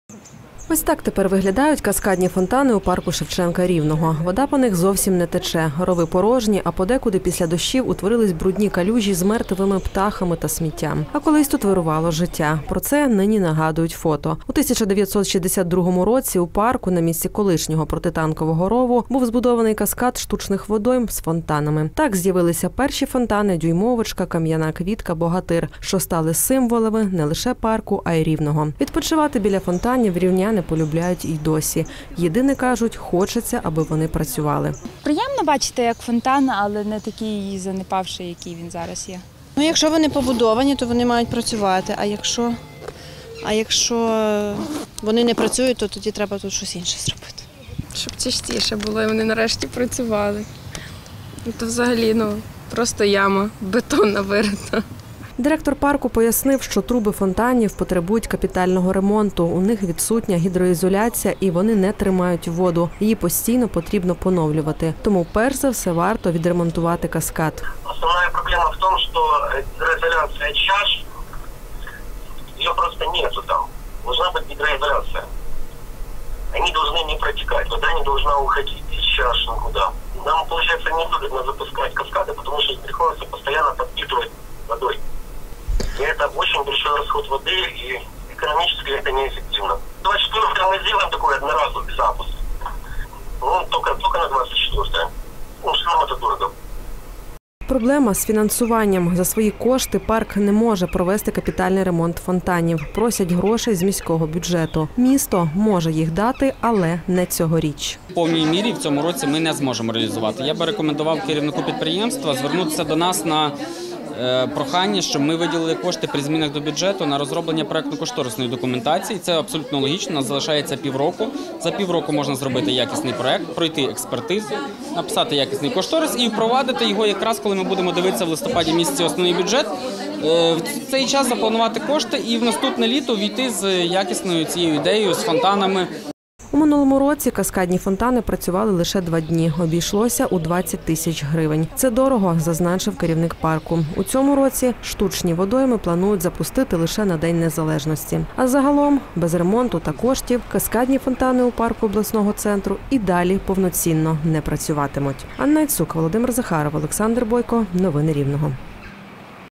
"Дюймовочка" без води: чому у парку Рівного не працюють каскадні фонтани?
У рівненському парку Шевченка не працюють каскадні фонтани. Штучні водойми у цьому році не запускали. Минулоріч лише на два дні, бо дорого. Улюблене місце відпочинку рівнян занепадає. Люди нарікають на неробочі фонтани і ностальгують, як вони працювали десятки років тому.